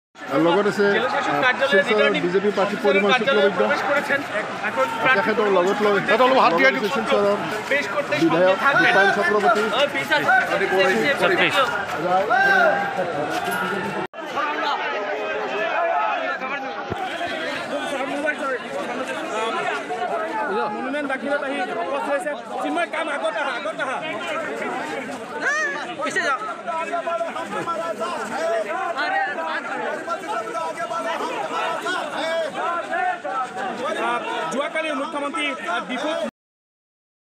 Parimal Suklabaidya files nomination from Assam’s Silchar LS seat
Video Report | Assam Chief Minister Dr. Himanta Biswa Sarma accompanied Parimal Suklabaidya, who filed his nomination for contesting from the Silchar Lok Sabha seat on Wednesday. BJP state president Bhabesh Kalita, Silchar MP Dr Rajdeep Roy, Silchar MLA Dipayan Chakraborty, Lakhipur MLA Kaushik Rai and other BJP leaders also walked in the rally, echoing support for Suklabaidya.